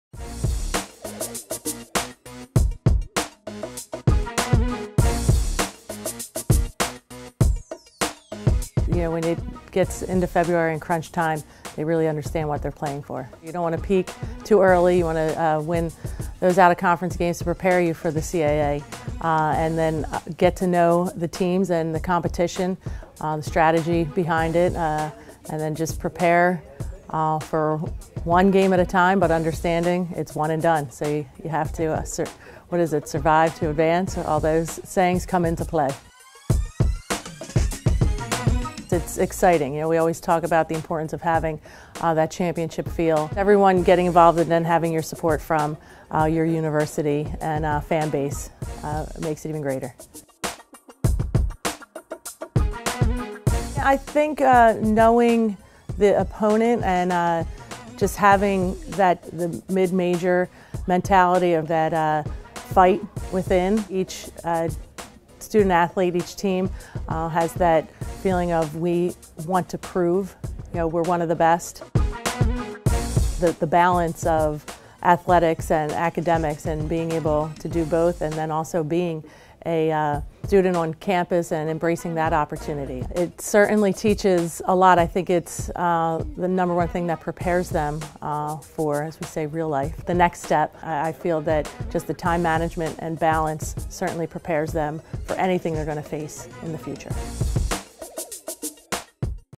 CAAHoops OVERTIME: Drexel's Denise Dillon
At the 2015 CAAHoops Media Day. CAA.tv sat down with all 20 of our head coaches. Enjoy "CAAHoops OVERTIME"